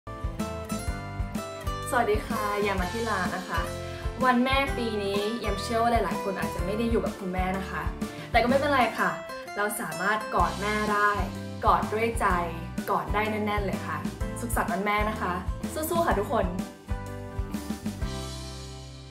Hug My Mom บอกรักแม่ด้วยอ้อมกอด | แยม มทิรา | Ch3Thailand
เนื่องในเทศกาลวันแม่แห่งชาติ แยม มทิรา ก็ขอเชิญชวนให้แฟนคลับทุกท่าน ออกมาแสดงความรักกับคุณแม่ และคนในครอบครัวของท่าน ด้วยกิจกรรม "Hug My Mom บอกรักแม่ด้วยอ้อมกอด แบบ New Noemal"